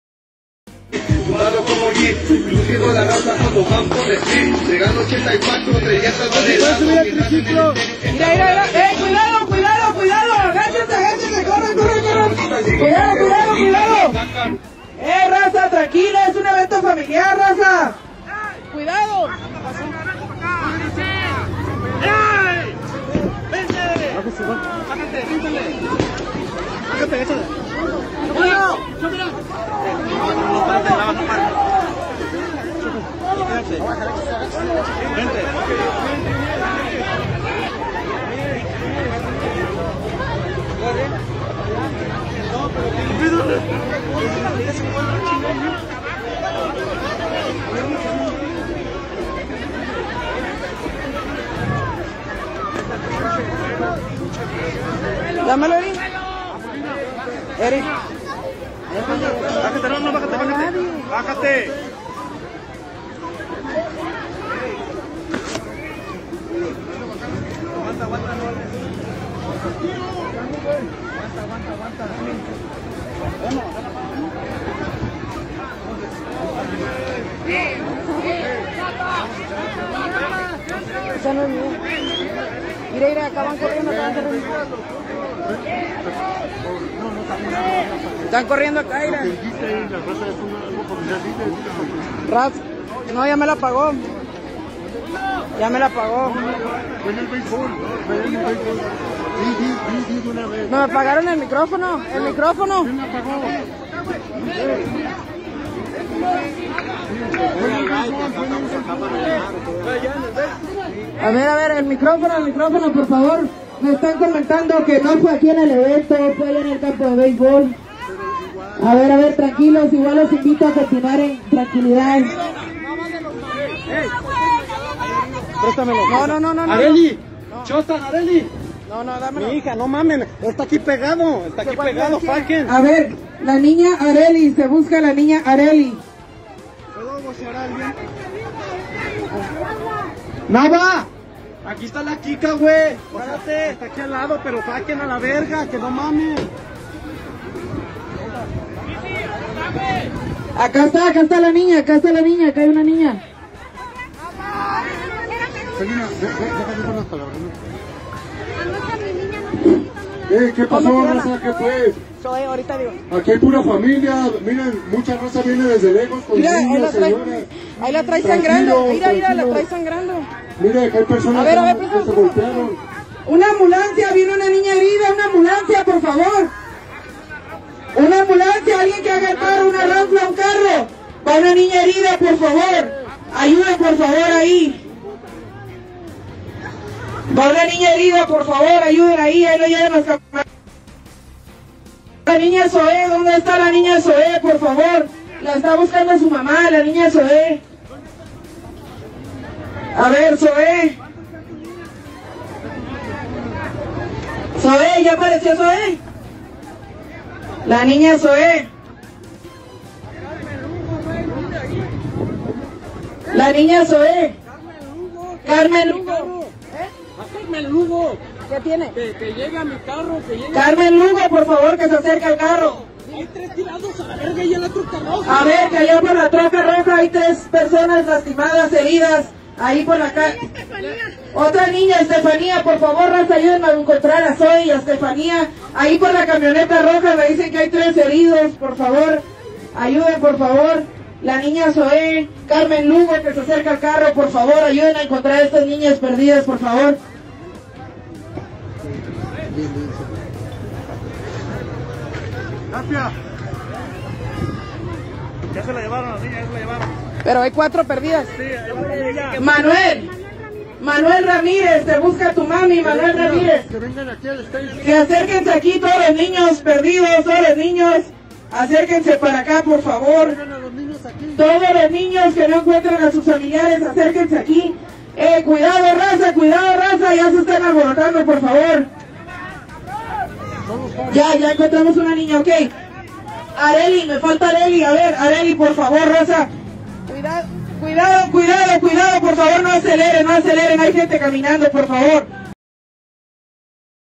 Apaseo el Grande Balacera en Auto Show
2 muertos y una menor herida